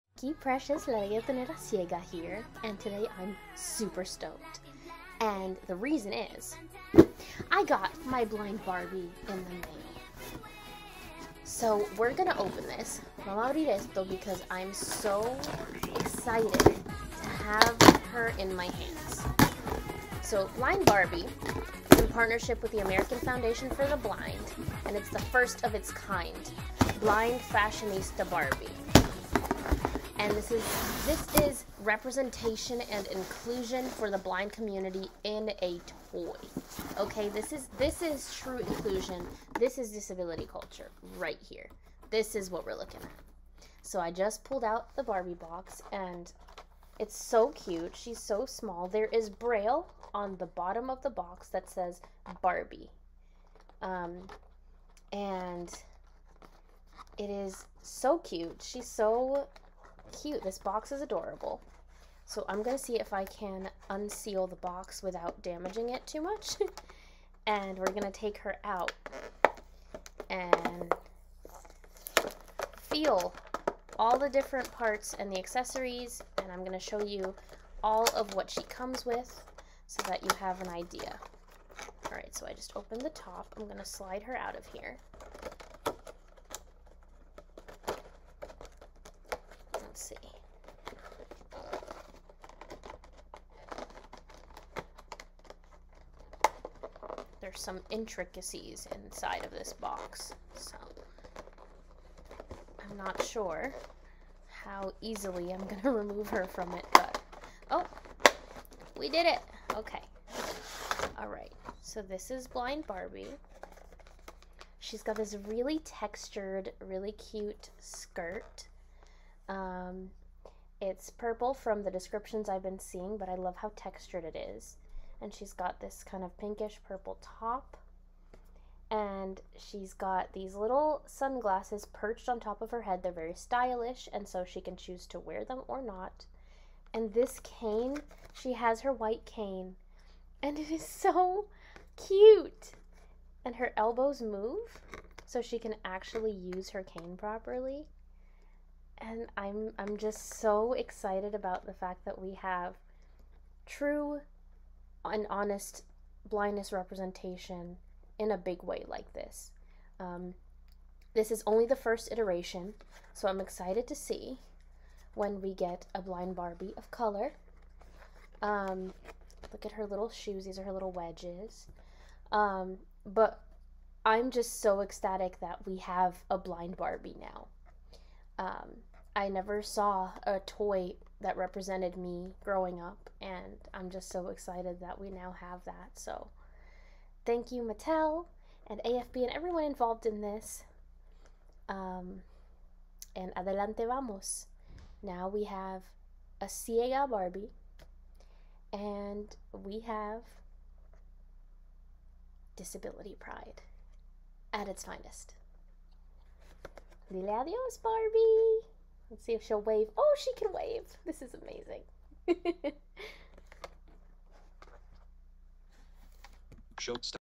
Unboxing Blind Barbie!!!
Unboxing blind Barbie!!! I'm usually more articulate, but the excitement took my words away. Cute seemed to be the only vocab word I could muster. Hahaha! LaReggaetoneraCiega BlindBarbie DisabilityCulture DisabilityPride 👠🩷👩‍🦯 @barbie